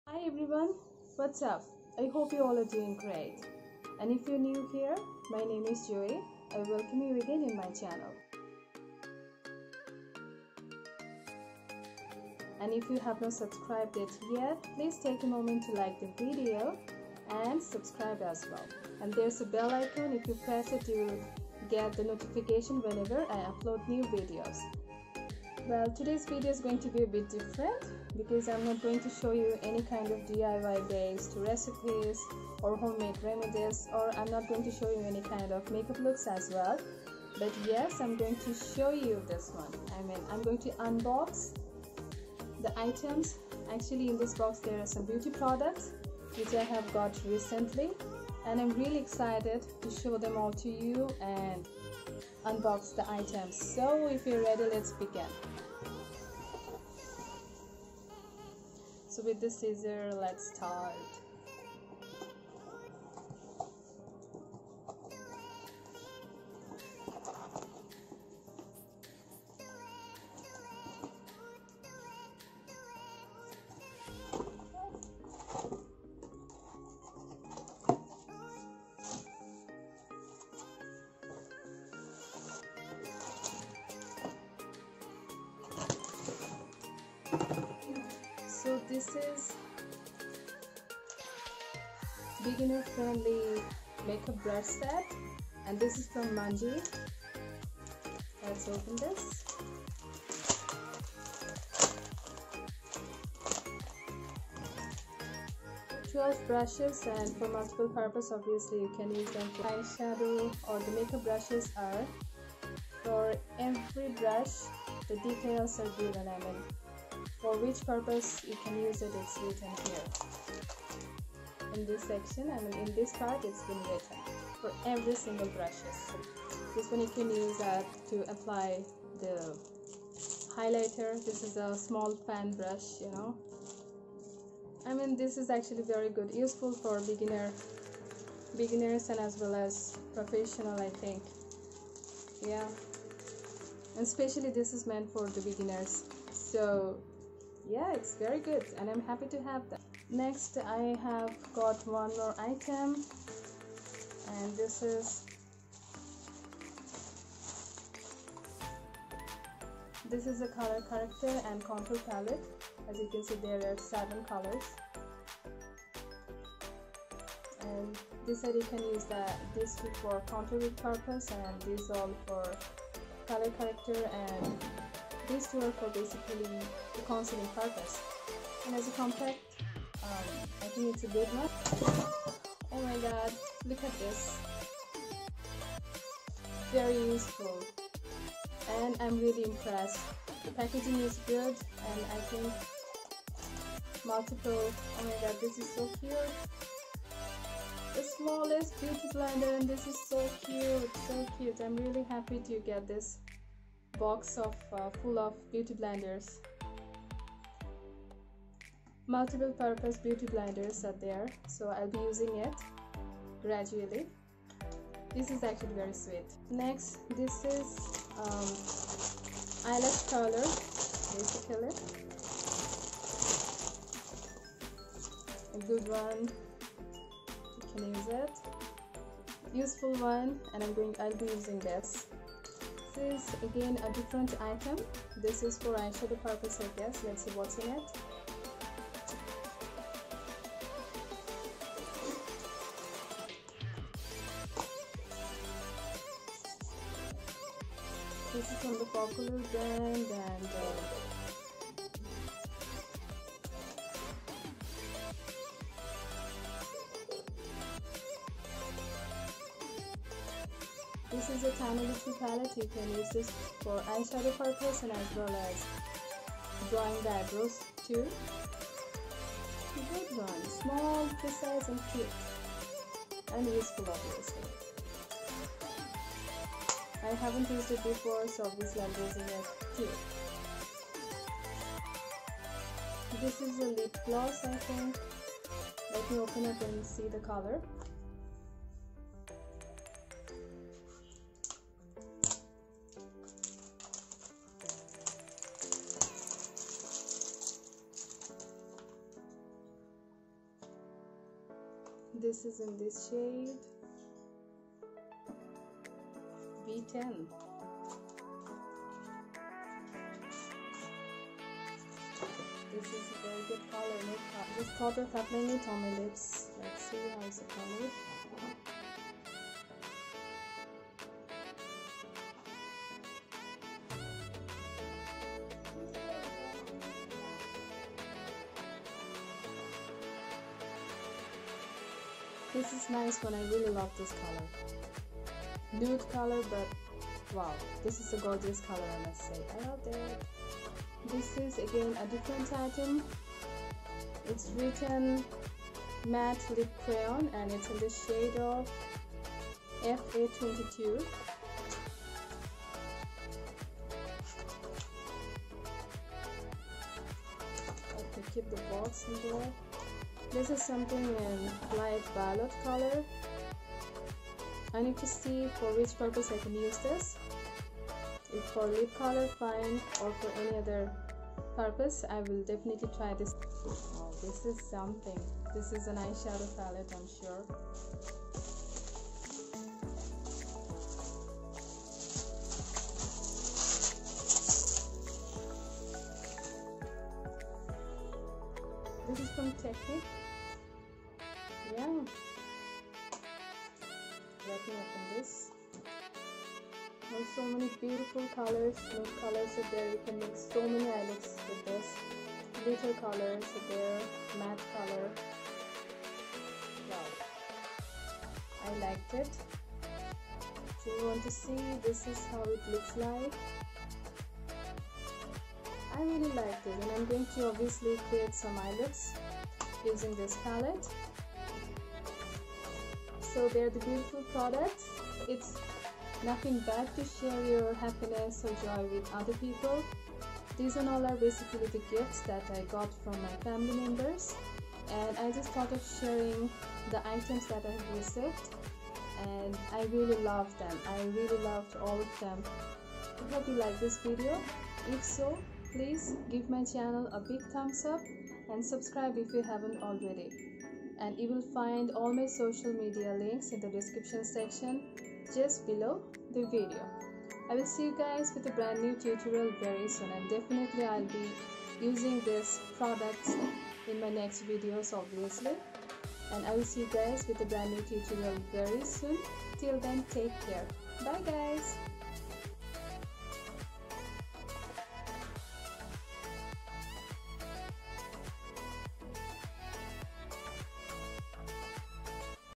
What's Inside ( Unboxing)|Drugstore Makeup Kits 💞
Hello everyone, hope you all are in sound health. This is my first Unboxing video 💕Well,I got them as a gift from my family members 🥰